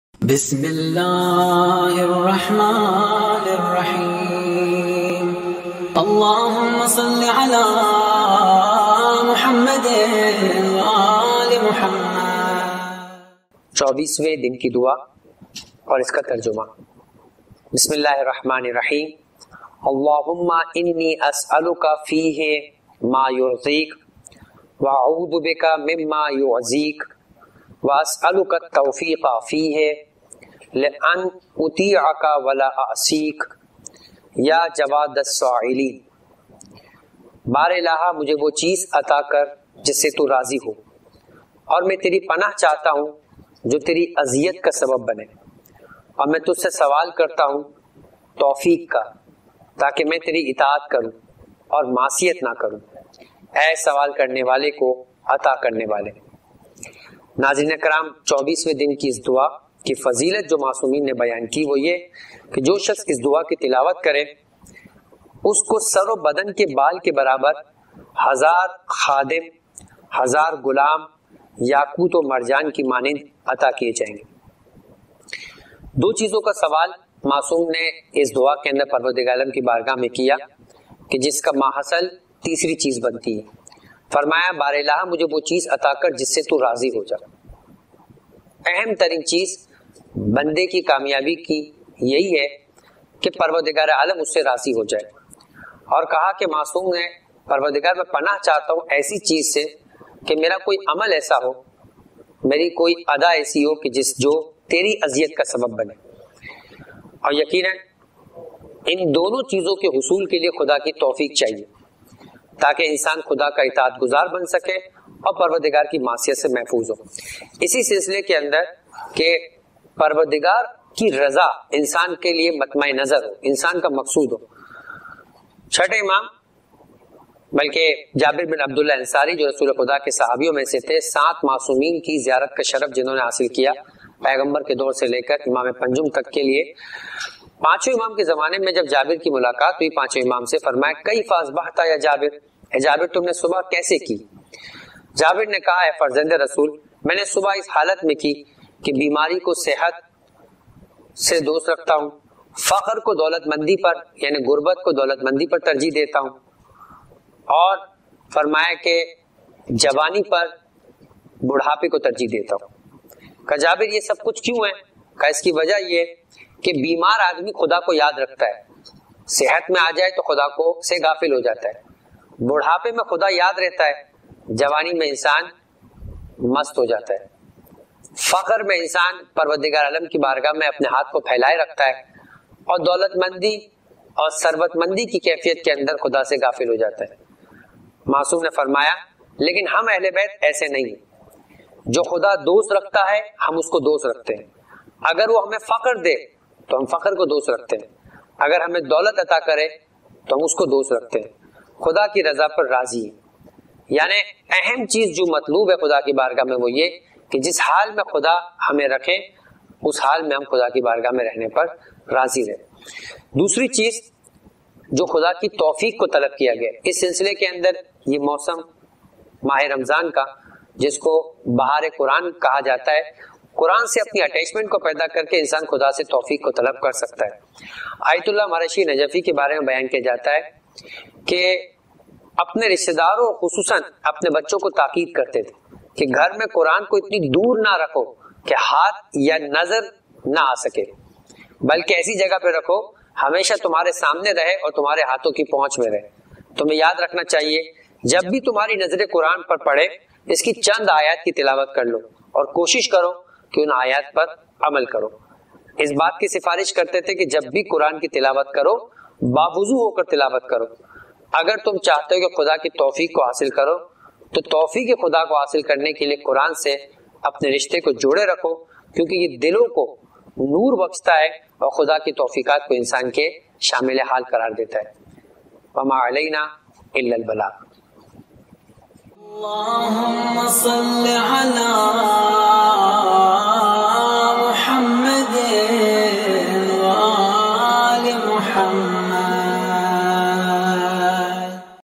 Mah-e-Ramzan Ke Chobeeswain Din Ki Dua | Maulana Qamer Ali Lilani [Esal e Sawab Committee]
Mah-e-Ramzan kai Chobeeswain din ki Dua, Tarjuma aur Tafseel by Maulana Qamer Ali Lilani